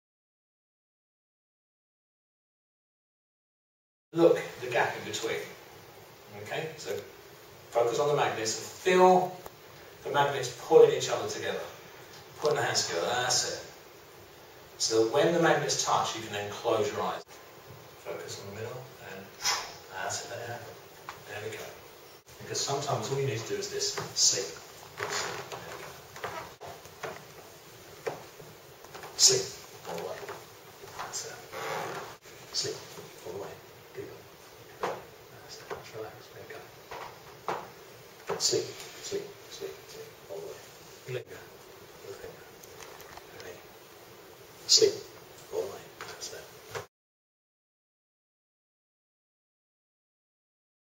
Hypnosis Rapid Inductions Number 2
The first morning of the Aventesi Certification Hypnosis training in London with Steve Crabb demonstrating with students a variety of instant hypnotic inductions